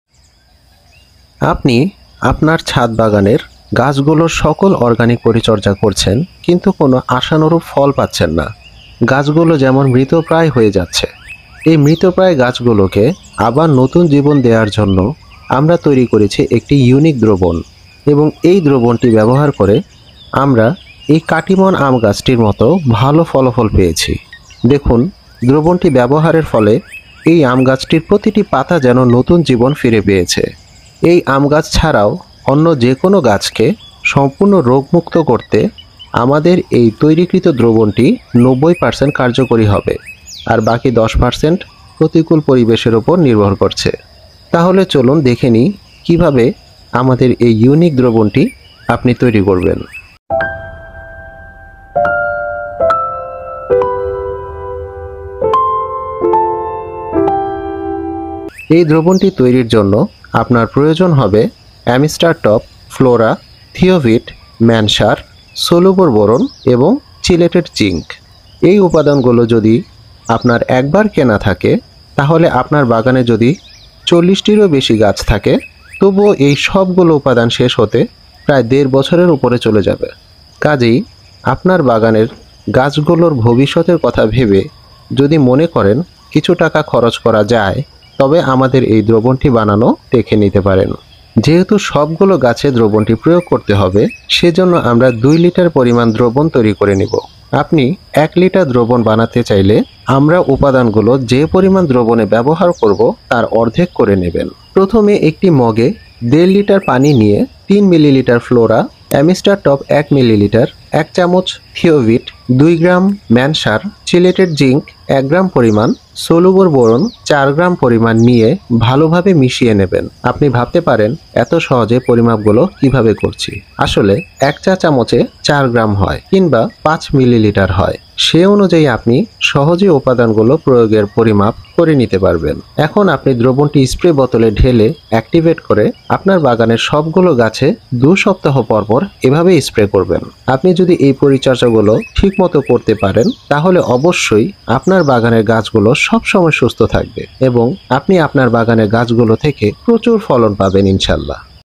গাছের যত্নের নতুন দিগন্ত: গাছের জন্য সবচেয়ে কার্যকরী দ্রবণ
ছাদ বাগানে গাছগুলোকে সুস্থ রাখা বেশ চ্যালেঞ্জের কাজ, তবে সঠিক যত্ন এবং পদ্ধতি মেনে চললে সহজেই তা সম্ভব। এই ভিডিওতে, আমরা ছাদ বাগানের গাছগুলোকে সুস্থ ও সবল রাখার জন্য এমন একটি কার্যকরী দ্রবণ বানিয়ে দেখিয়েছি যা দু সপ্তাহ পর পর ব্যবহার করলে আপনাকে আপনার ছাদ বাগানের গাছগুলো নিয়ে বেশি ভাবতে হবেনা।

তবে দ্রবণটি ব্যবহারের পাশাপাশি কিছু কাজ আপনাকে অবশ্যই করতে হবে সেগুলো নিচে দেয়া হলো:

১) আপনার গাছের টব গুলোতে পর্যাপ্ত পরিমাণ পানি নিষ্কাশনের ব্যবস্থা করুন।
২) আপনার ছাদ বাগানকে সব সময় পরিষ্কার পরিচ্ছন্ন রাখুন এটা খুবই গুরুত্বপূর্ণ।
৩) গাছে ফুল আসার আগ পর্যন্ত জৈব সার (১ মুঠ), নিম খইল(১ মুঠ), হারের গুড়া + শিংকুচি মিশিয়ে (১ মুঠ), সরিষার খইল গুড়া (অর্ধেক মুঠ) ১০ গ্যালন সাইজের টবের হিসেবে ১ মাস পর পর দিয়ে দেবেন।
৪) গাছে ফুল এসে গেলে এককালীন সার হিসেবে, এন পি কে সার দুই চা চামচ পরিমাণ, জৈব সার ২ মুঠ, ম্যাগসার দেড় চা চামচ, হারের গুড়া ১ মুঠ, ১০ গ্যালন সাইজ টবের হিসেবে দিয়ে দেবেন।
৪) এই সব পরিচর্যার পর আপনি আমাদের তৈরি কৃত দ্রবণটি ব্যবহার করলে খুব ভালো ফলাফল পাবেন ইনশাল্লাহ।